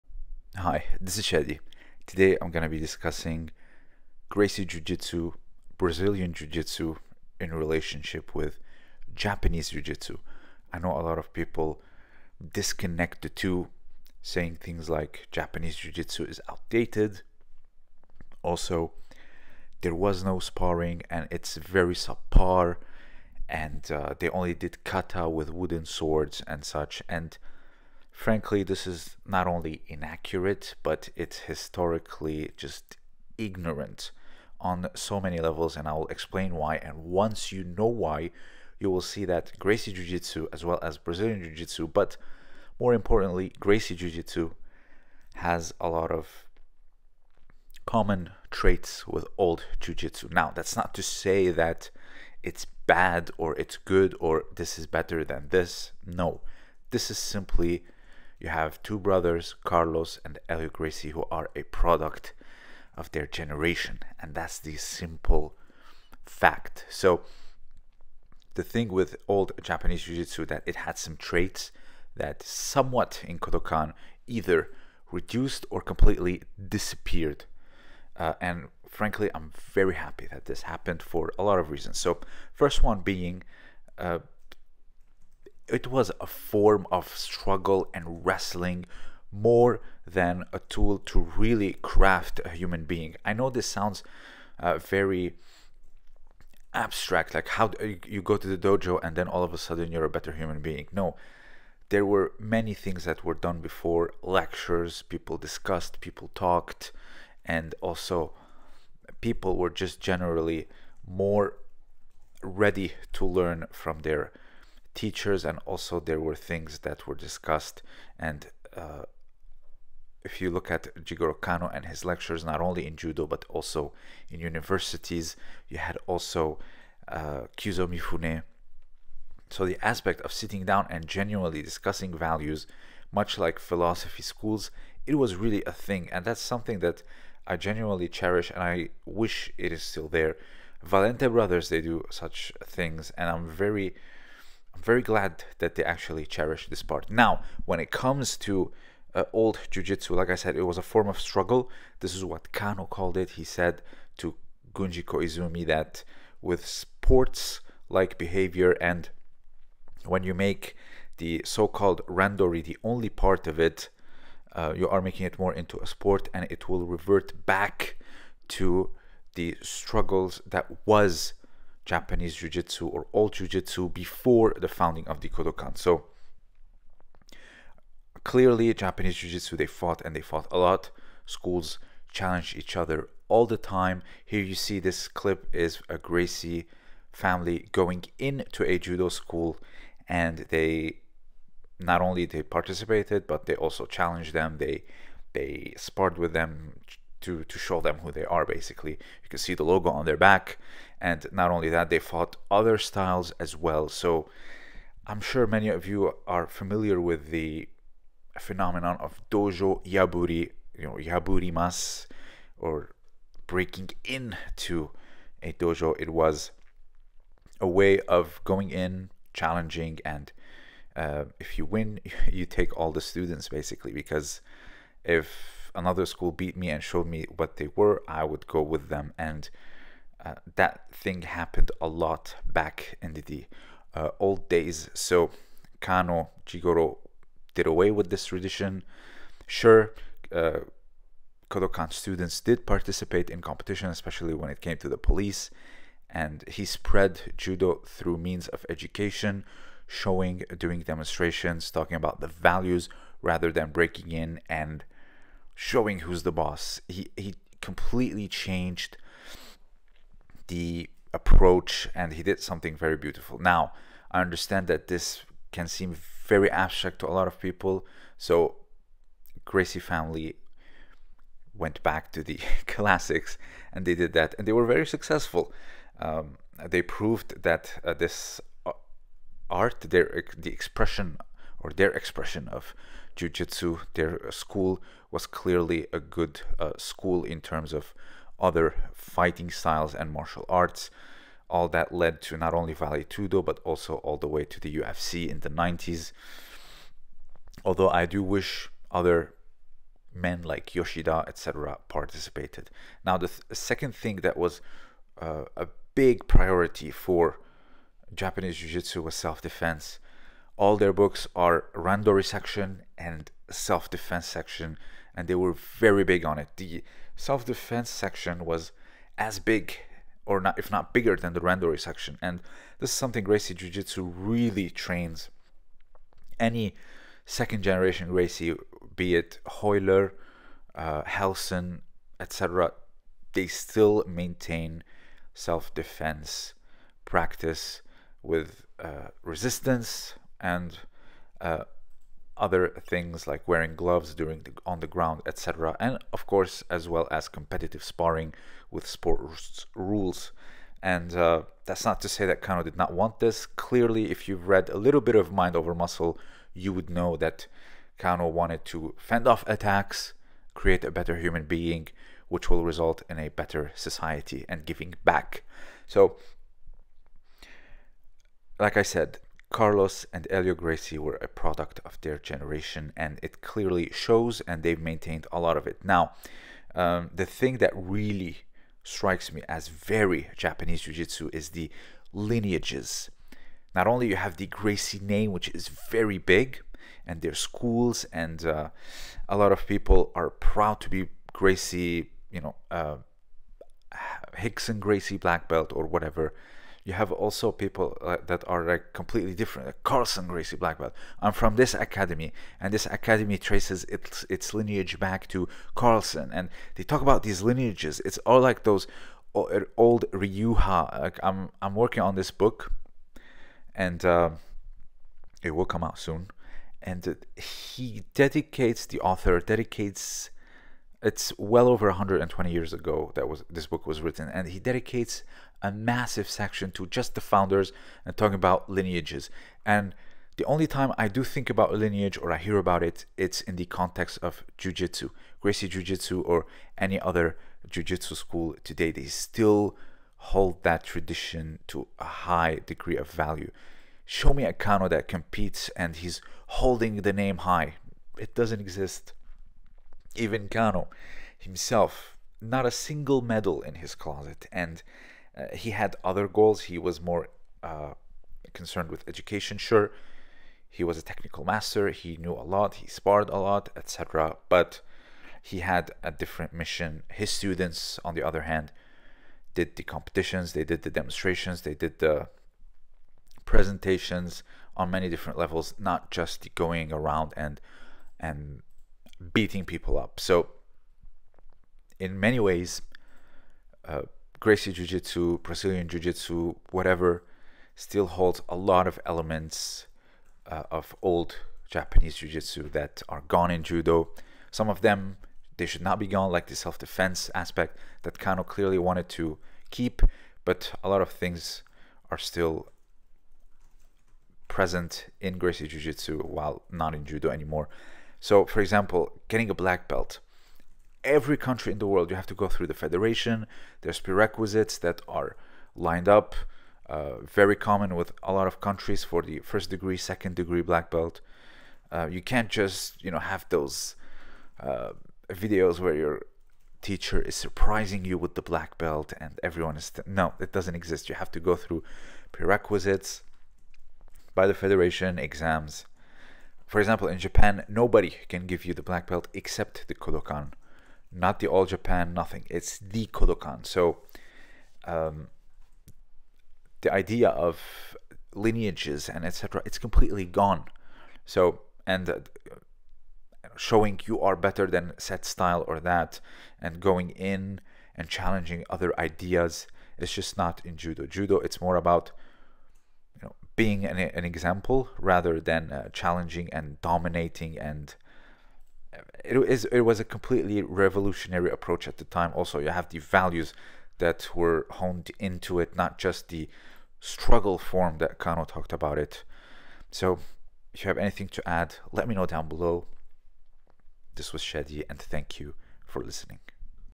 Gracie jujutsu has more in common with Japanese jujutsu than you think
This video discusses the subtle and common traits between Gracie jujutsu and Japanese jujutsu.

My book "Une silhouette de mots":